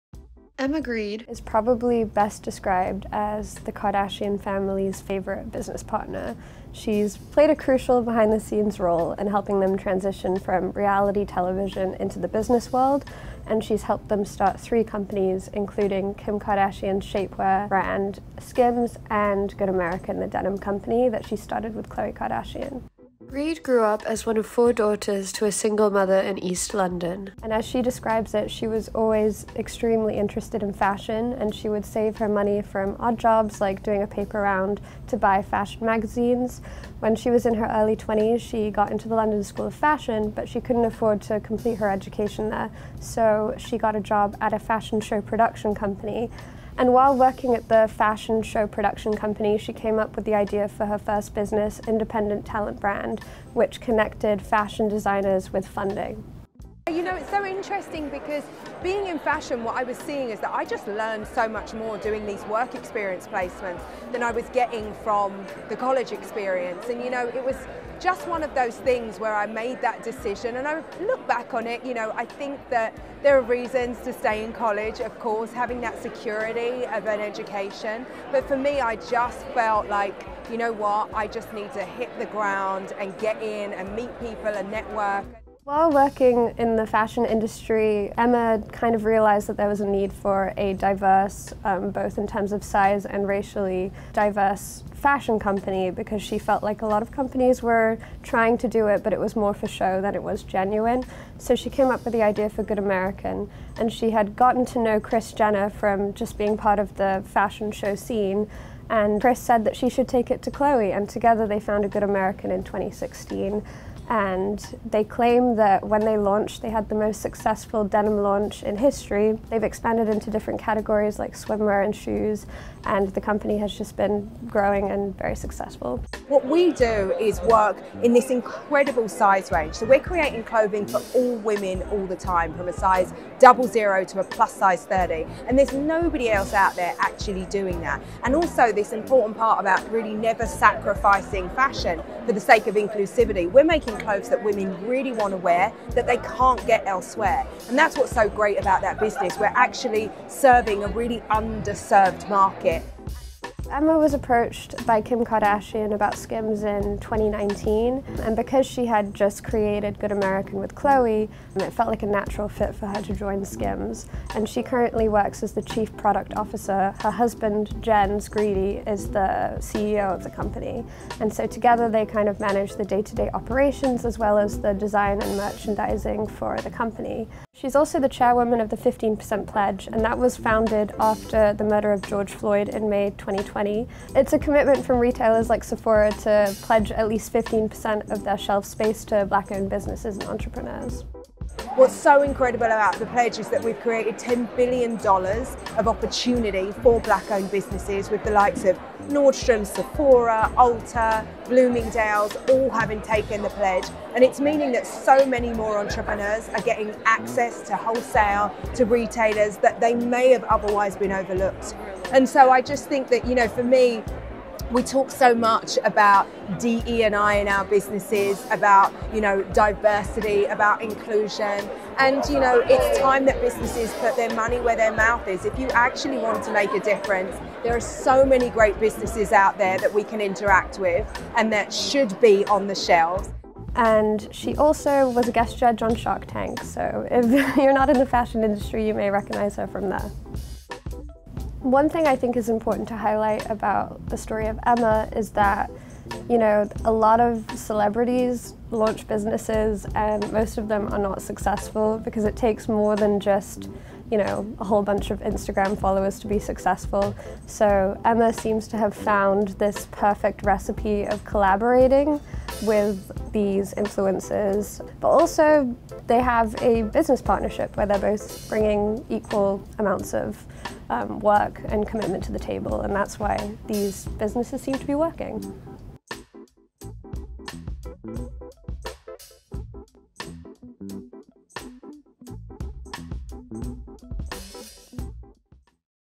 A mulher que fez fortuna ajudando as Kardashians a construir império
Emma Grede, empresária de 39 anos, foi uma das responsáveis pela transição das estrelas de reality Kim, Khloé e família para o empreendedorismo. Conheça a história dela.